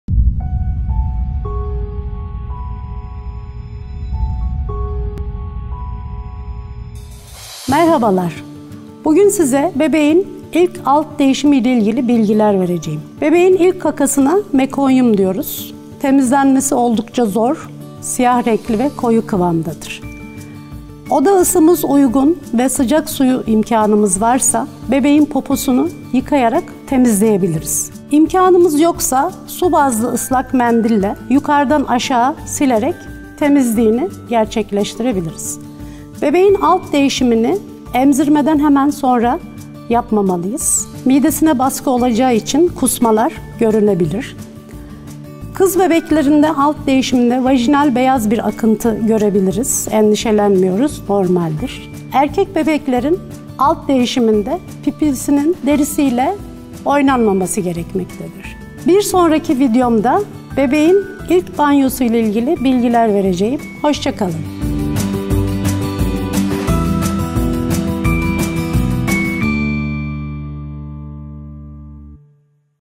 Bebeğinin İlk Alt Değişimi için Bilmen Gerekenler
Bebeklerin ilk alt değişiminde nelere ihtiyaç duyulur? Bu konuda nelere dikkat edilmelidir? 👀
İşte tüm bu soruların için Elika yanında! 🤗 Elika ailesinden Ebe Fatma Doğan bebeklerin ilk alt değişimi ile ilgili bilgiler veriyor.